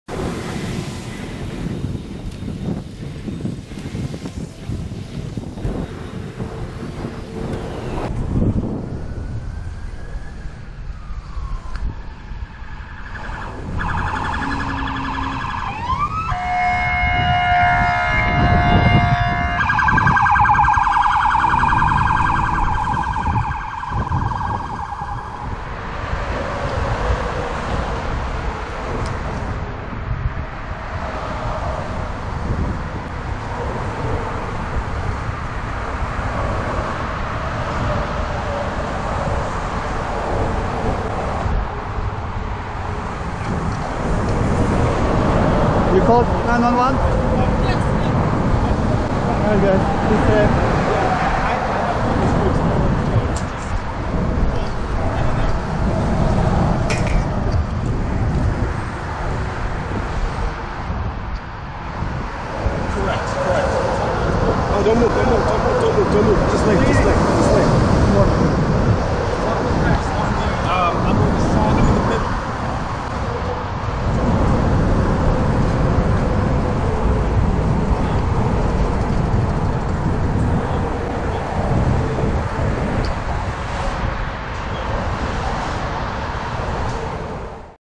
Hot day ride NYC June 23rd, 2024 highlights
00:00 Hot day ride NYC June 23rd, 2024 highlights

The temperature is 95F at 2pm.
I am going up the ramp on the Roosevelt island bridge.
Slowing down as I hear incoming sirens on two ways road...
An incoming truck moves against traffic and cops coming on the same lane!
Is it a chase?!
I am safely out of their way.
7 minutes later, I am on Queensborough 59th bridge going to Manhattan.
The traffic is light on the bike lanes of the bridge at this time of the hot day.
Oh-ho, something happened in front...
Looks like a delivery guy on e-bike is down...and a gray city bike is down too.
Sh!t happens all the time even on empty roads.
Snap back to reality - cyclists are the most vulnerable among all on the road.
Be safe